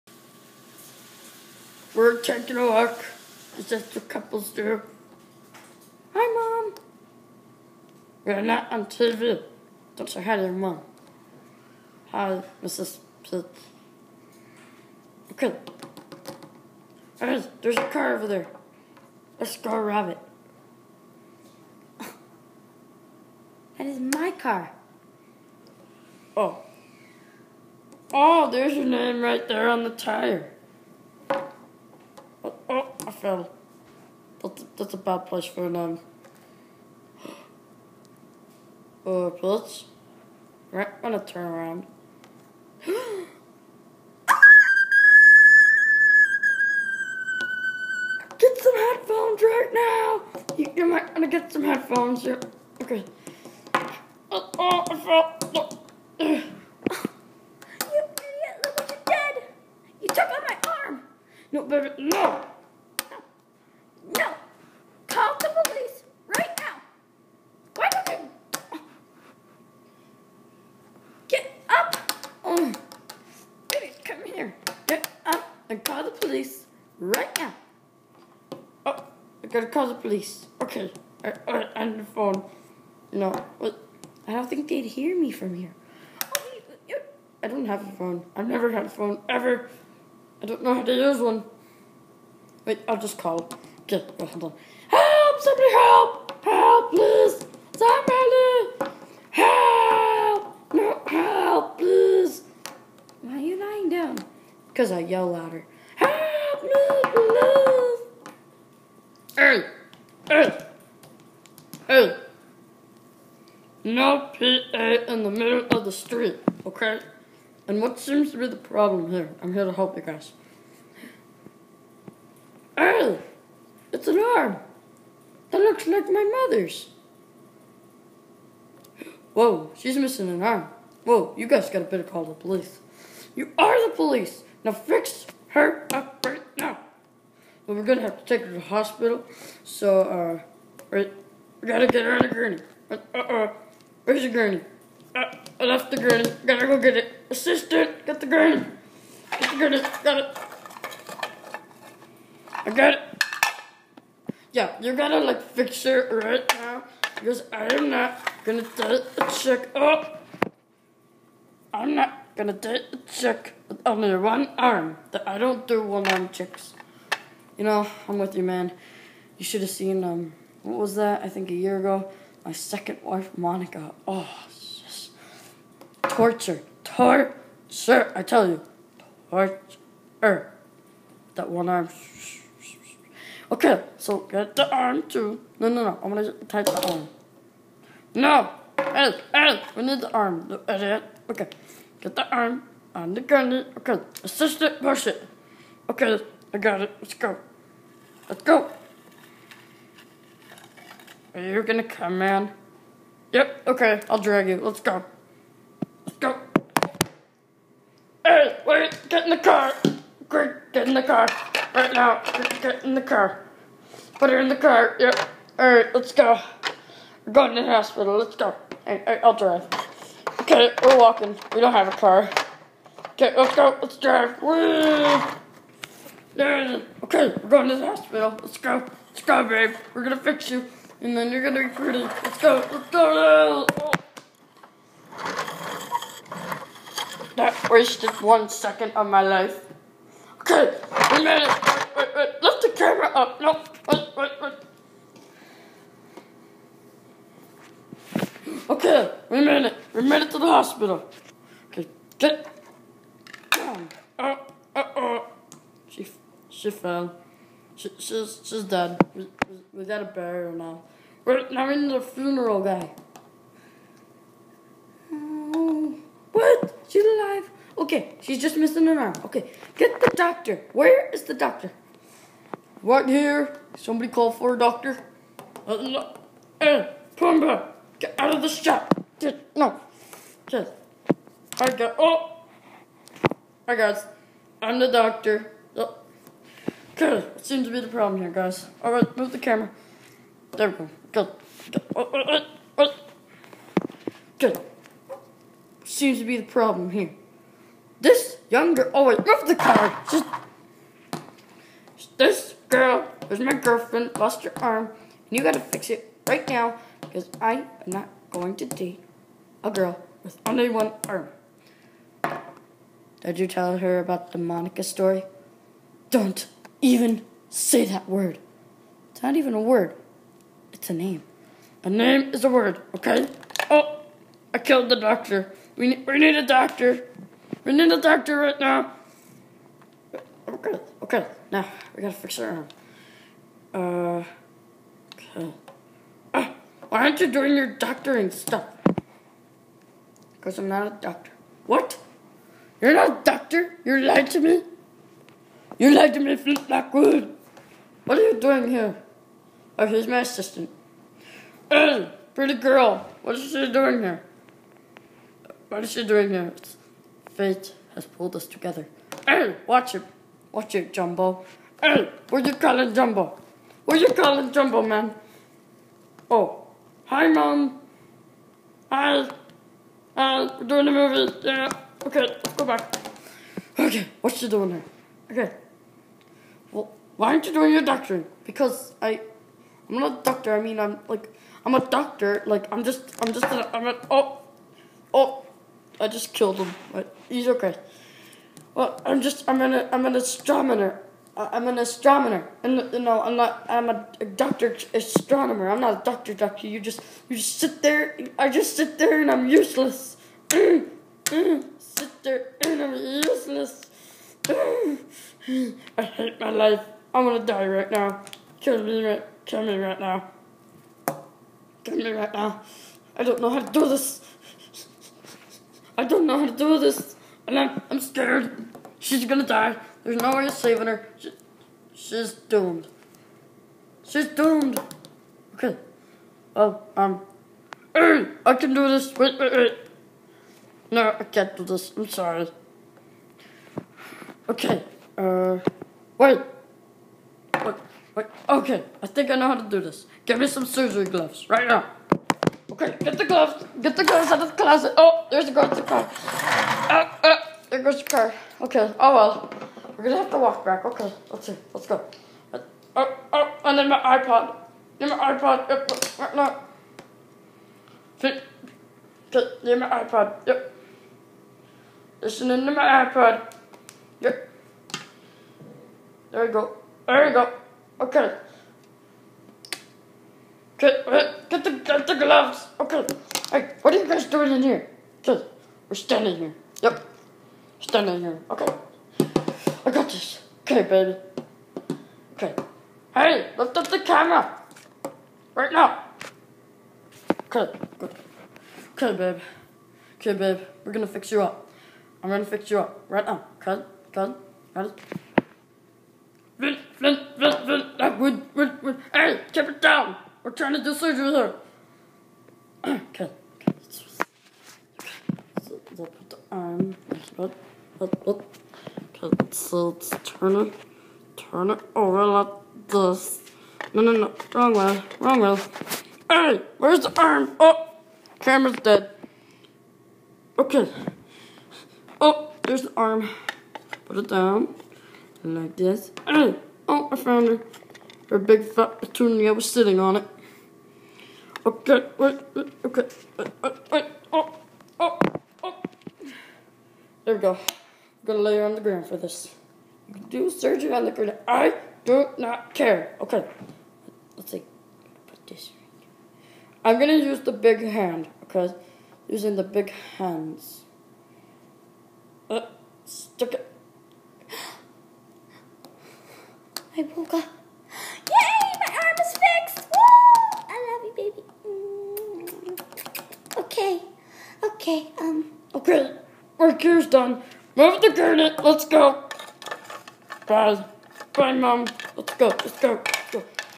Peach's Arm Surgery
Peach is with her boyfriend and loses her arm. Will it be fixed?